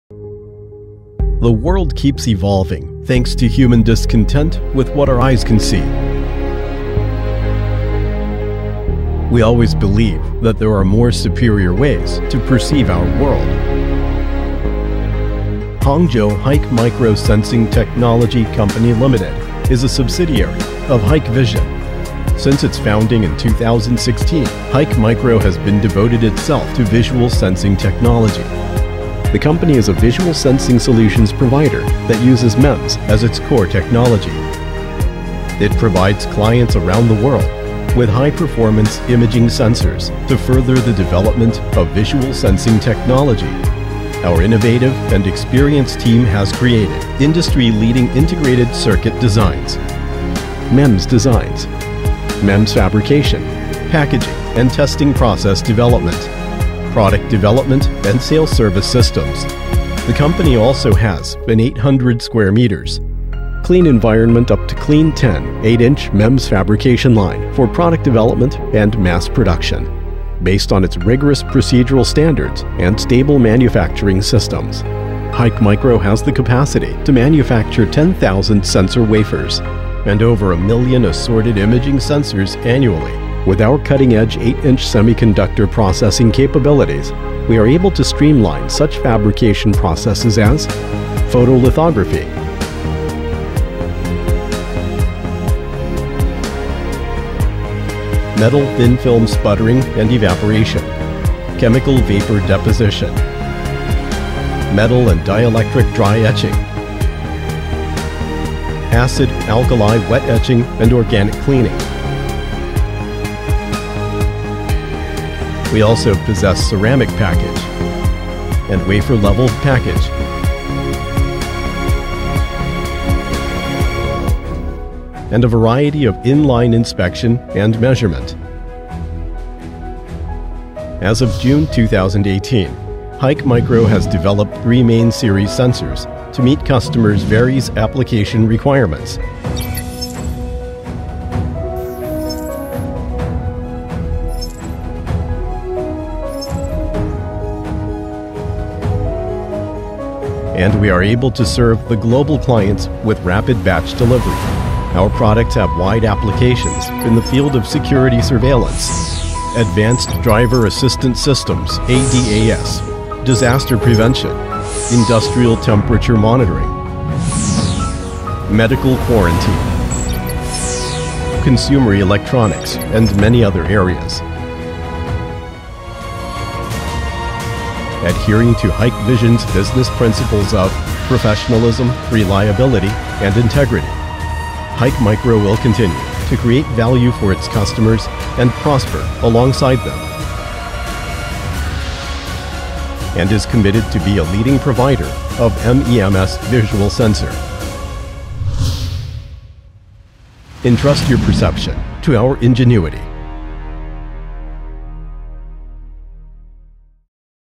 Hikmicro company video
Hangzhou Hikmicro Sensing Technology Co., Ltd, as a visual sensing solutions providers, is established to design, manufacture and provide the greatest thermal imaging sensors.